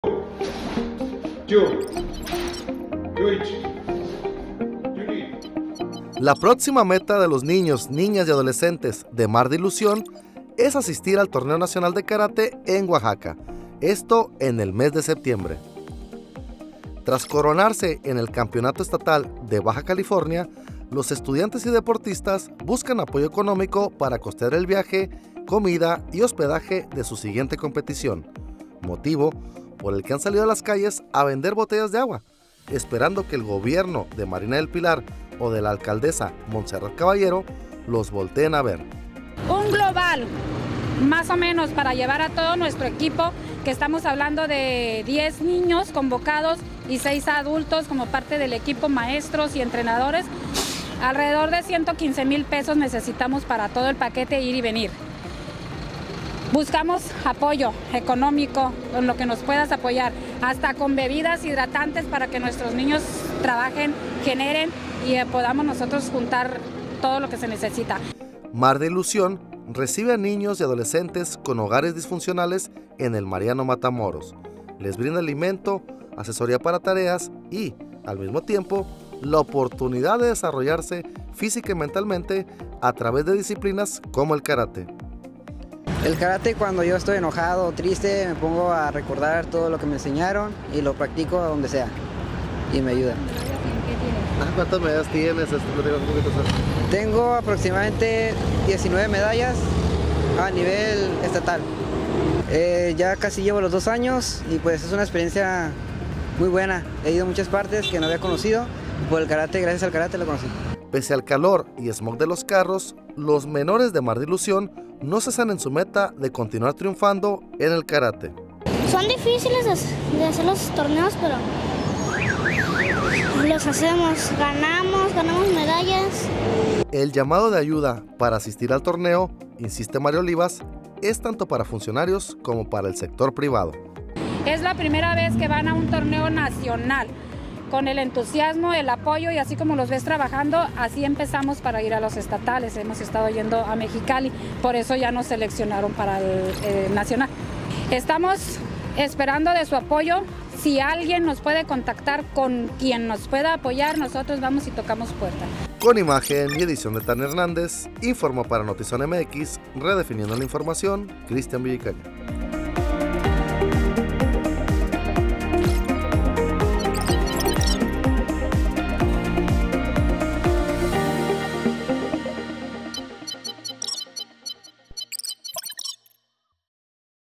Menores buscan apoyo para torneo nacional de karate - ZONA MX
Tras coronarse en el campeonato estatal de Baja California, los estudiantes y deportistas buscan apoyo económico para costear el viaje, comida y hospedaje de su siguiente competición